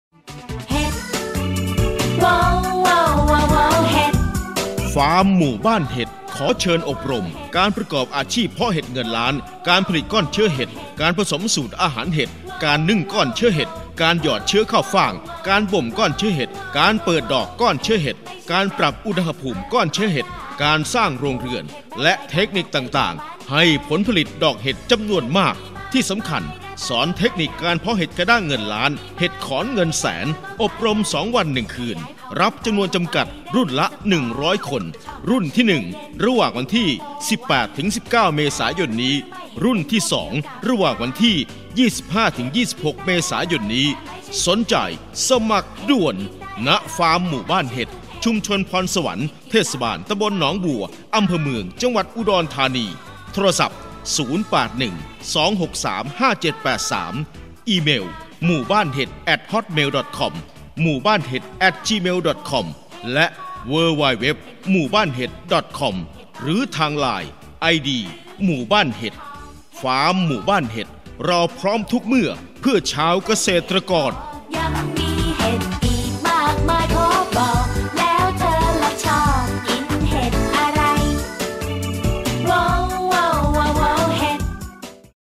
เชิญอบรมการเพาะเห็ดเงินล้าน ฟาร์มหมู่บ้านเห็ด อุดรธานี
ฟาร์มหมู่บ้านเห็ด เชิญอบรม "การประกอบอาชีพเพาะเห็ดเงินล้าน" การผลิตก้อนเชื้อเห็ด การผสมสูตรอาหารเห็ด การนึ่งก้อนเชื้อเห็ด การหยอดเชื้อข้าวฟ่าง การบ่มก้อนเชื้อเห็ด การเปิดดอกก้อนเชื้อเห็ด การสร้างโรงเรือน การปรับอุณหภูมิ และ เทคนิคต่าง ๆ ให้ผลิดอกจำนวนมาก ๆ ที่สำคัญ "สอนเทคนิคการเพาะเห็ดกระด้างเงินล้าน เห็ดขอนเงินแสน" อบรม 2 วัน 1คืน รับจำนวนจำกัด รุ่นละ 100 คน รุ่นที่ 1 ระหว่างวันที่ 18-19 เมษายน 2558 และ รุ่นที่ 2 ระหว่างวันที่ 25 - 26 เมษายน 2558 "สนใจสมัครด่่วน ณ ฟาร์มหมู่บ้านเห็ด ชุมชนพรสวรรค์ ทต.หนองบัว อ.เมือง จ.อุดรธานี โทร.